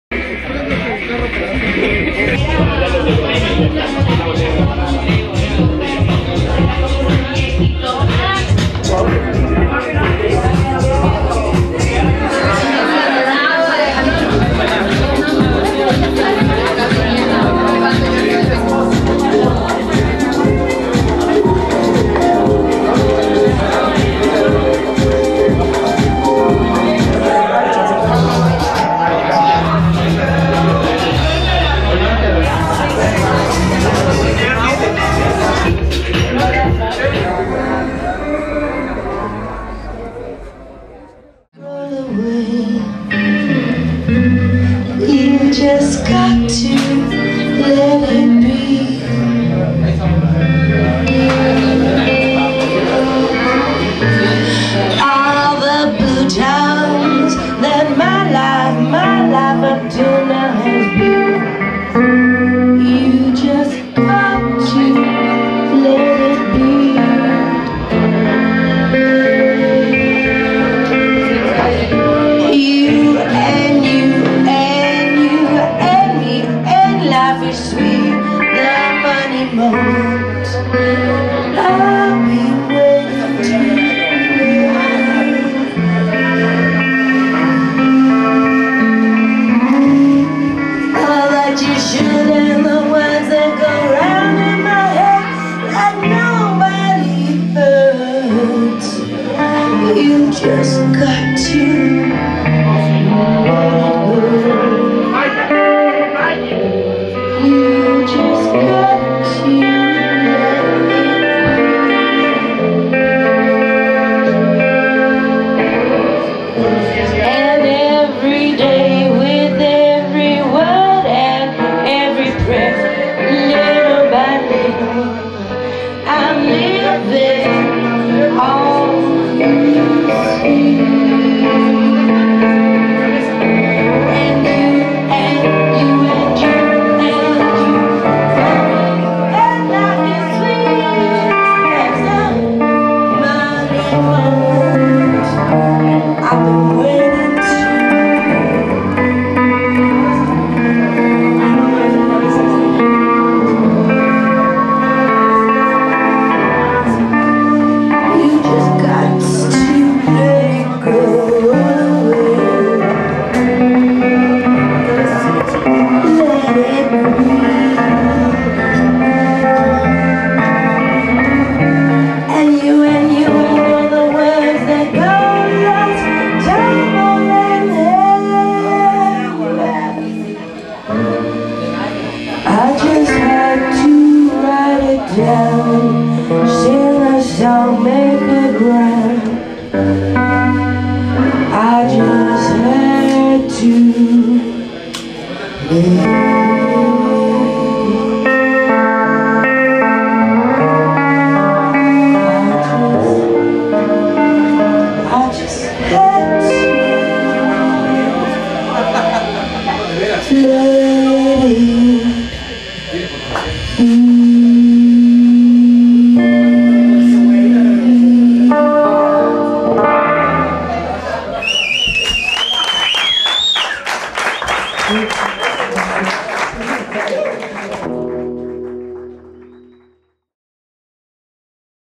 HOMENAJE en Jazzatlan - 04
HOMENAJE en Jazzatlan -
Jazzatlan, San Pedro, Cholula, Puebla, México
by cainKuri

HOMENAJE
son:
- Ana Chantalla Mattocks
- Saul Ibarra
- Juan carlos Negrete
- Ariam Ibarra

Ana Chantalla Mattocks, Saul Ibarra, Juan carlos Negrete, Ariam Ibarra